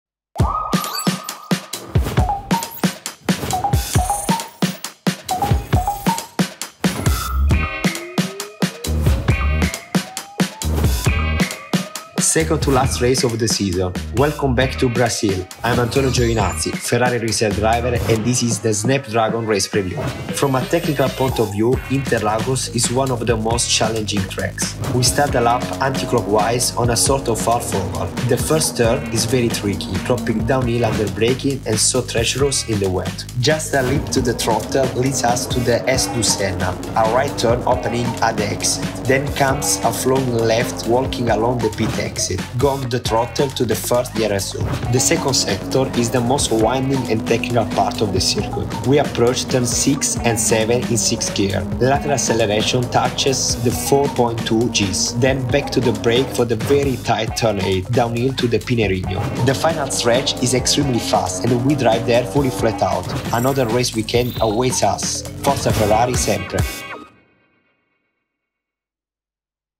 São Paulo Grand Prix Preview - Scuderia Ferrari 2022
Off to Brazil for the penultimate race of 2022 with @Anto_Giovinazzi and @Snapdragon 🙌

In this preview…
🔘 Interlagos Fact File 📖
🔘 Three Qs to Laurent Mekies 🏎️
🔘 Facts and Figures 📊